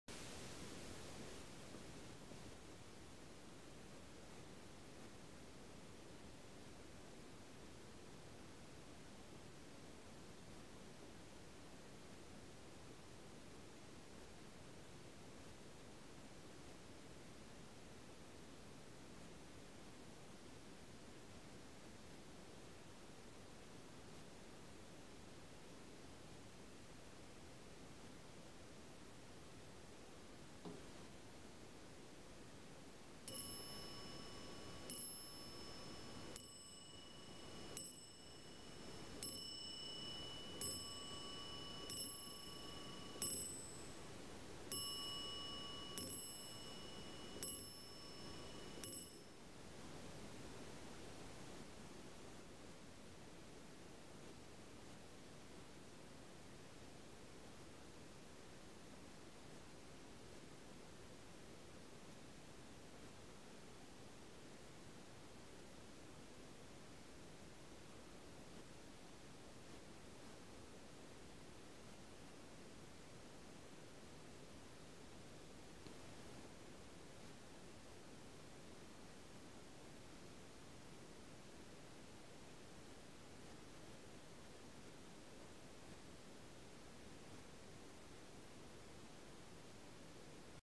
ancienne pendule a colonnes napoléon III mouvement de paris rare P AMIC japy fils
ancienne pendule a colonnes napoléon III mouvement de paris rare P AMIC japy fils en fonction avec son balancier, elle est a restaurer